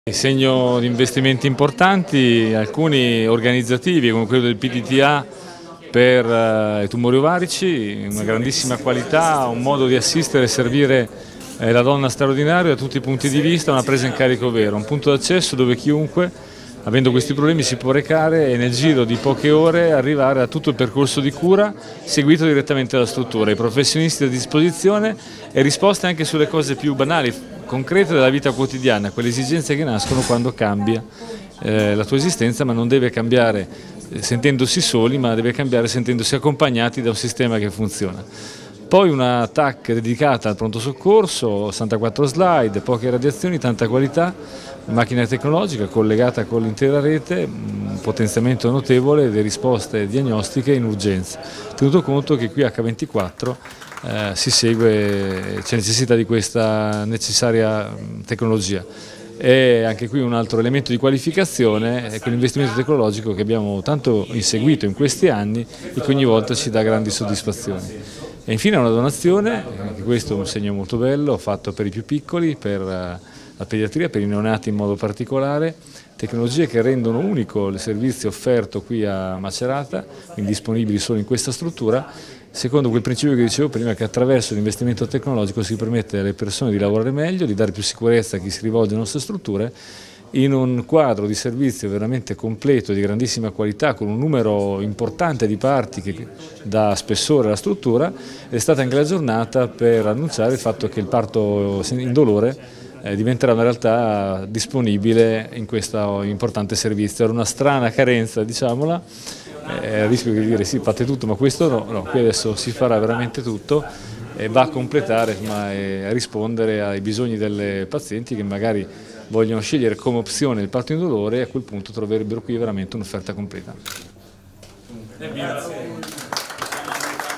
Tre inaugurazioni all'ospedale di Macerata
Ceriscioli: "Investimenti di qualità per una sanità di eccellenza al servizio di tutti".